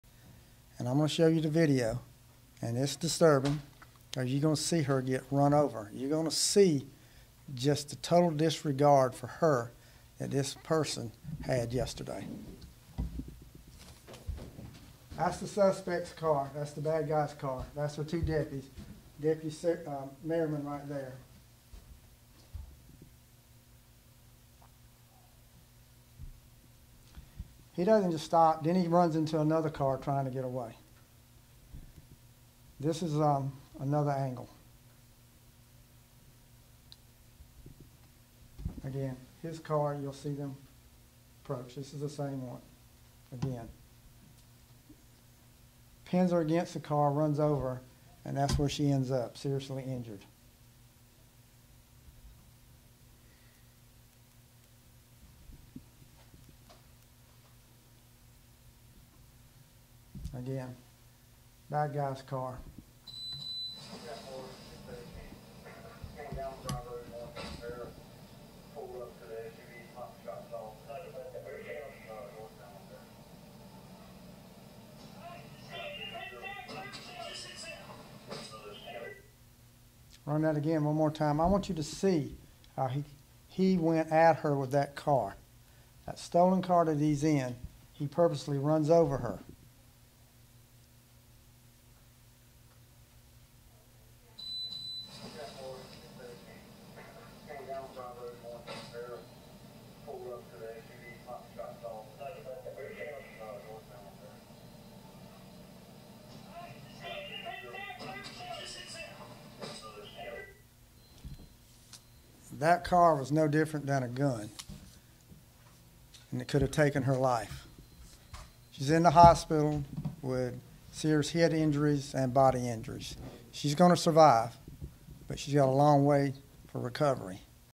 Richland County sheriff shows video of officer being hit by suspect's car
Deputy Sara Merriman was hit by a suspect's car Tuesday while on the job in Columbia.